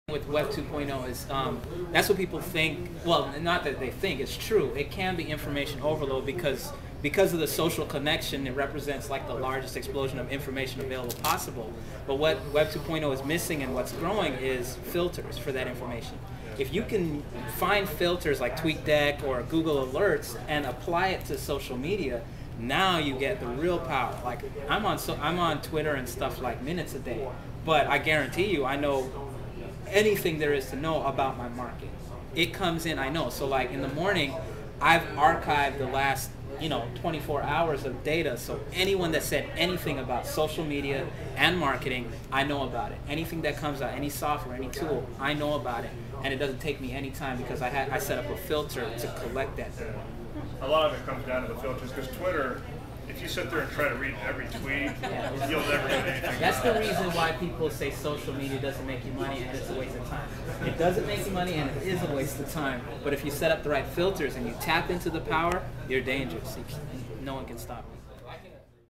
Why Social Media Doesn't Work For Most People
Why doesn't social media work for everyone who wants it to work for them? They don't know what myself and a small group of marketers know. Watch the video to find out the one thing you must have in place in order to leverage social media where it works for you rather than against you